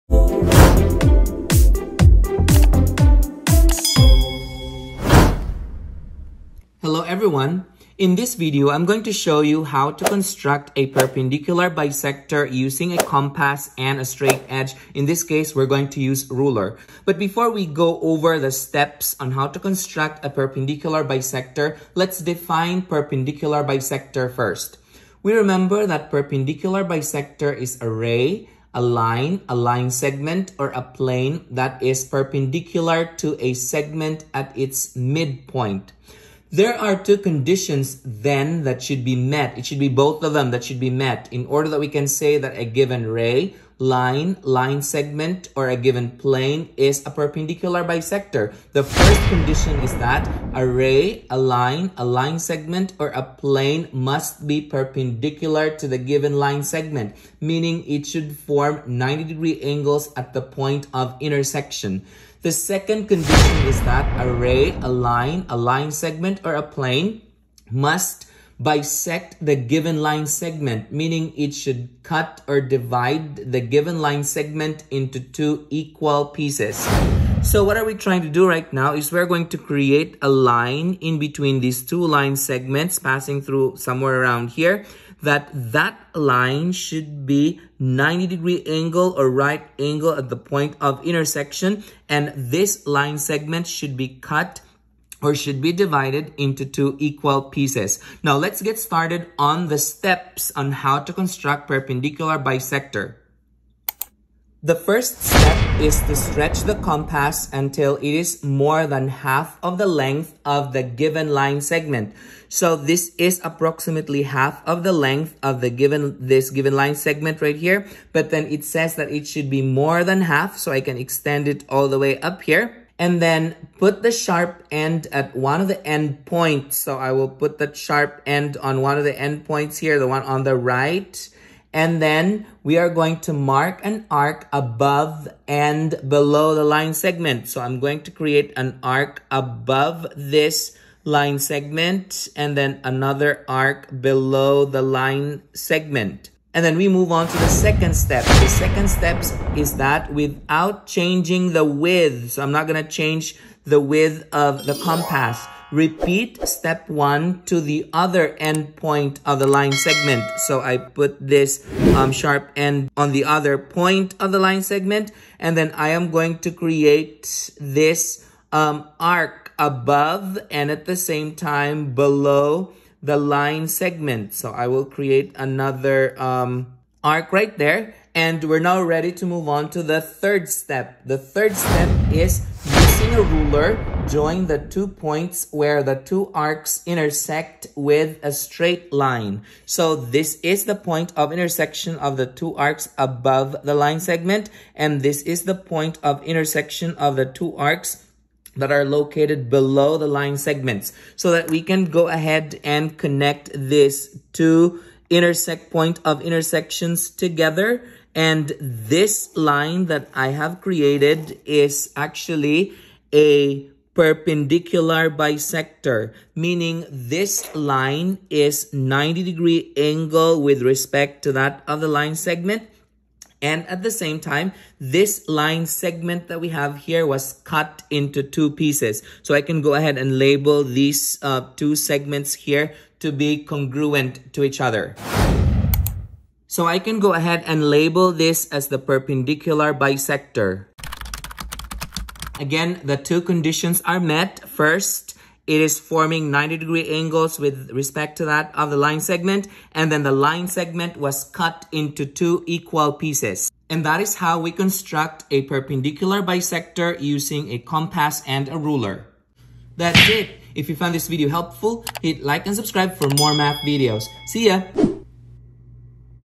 How to Construct a Perpendicular Bisector
This video will show the step-by-step method in constructing a perpendicular bisector of a line segment using compass and ruler